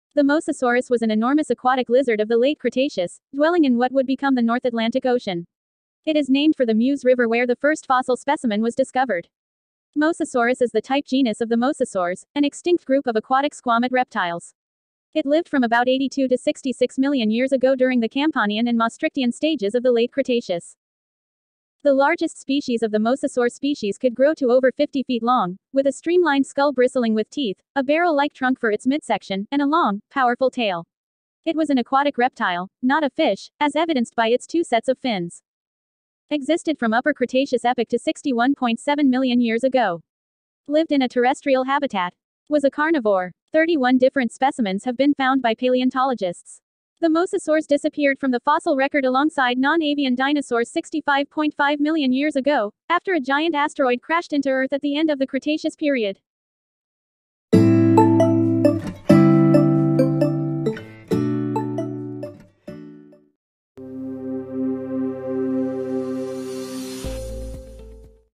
Meet the Mosasaurus: The Deadly Giant of Jurassic Seas|Not a lizard or Fish
In Badlands National Park, mosasaur fossils have been found in the Pierre Shale, a rock unit laid down in the Western Interior Seawa